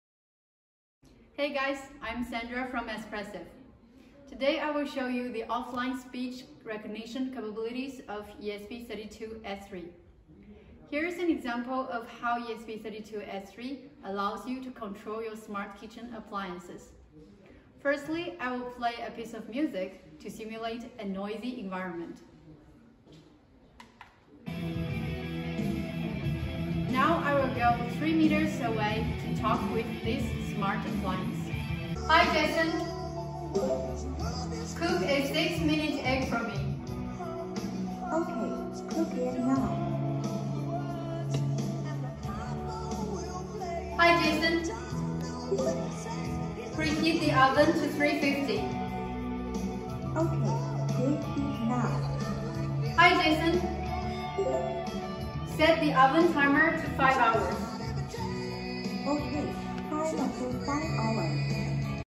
ESP32-S3 Offline Speech Recognition
Following our previous video about the online speech recognition capabilities of ESP32-S3, this video demonstrates the offline speech recognition capabilities of ESP32-S3, in a noisy environment. The ESP32-S3 microcontroller unit does not have any external DSP chips, and yet it features blind source separation (BSS), acoustic echo cancellation (AEC), and a wake-work engine (WWE). For more details, please contact our customer support team